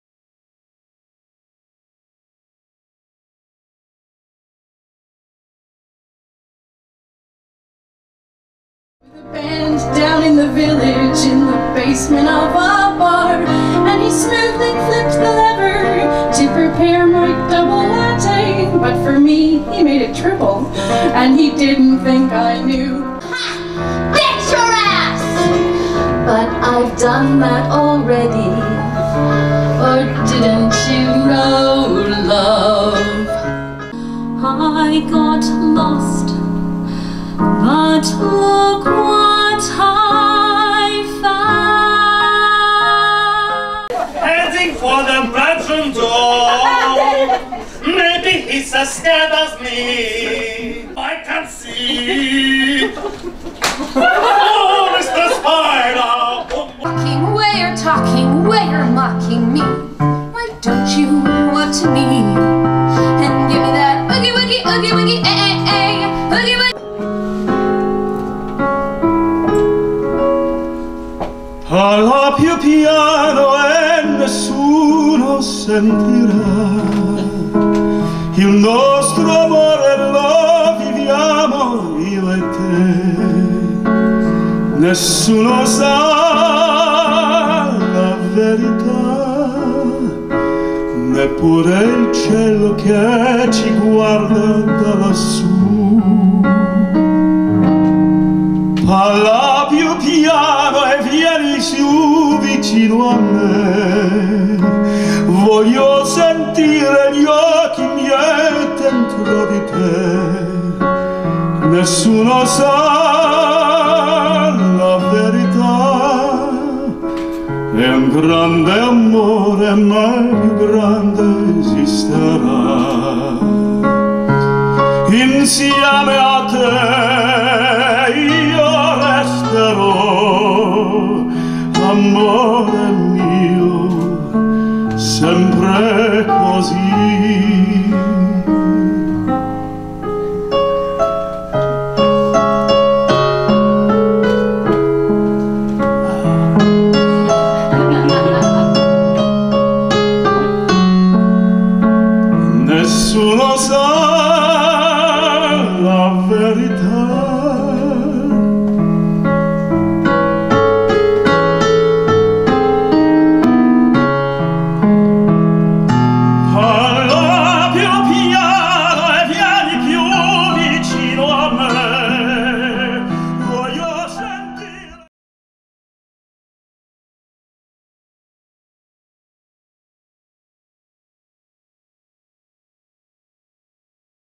The North American Actors Association
The North American Actors Association fund raising event at The Battersea Barge, London.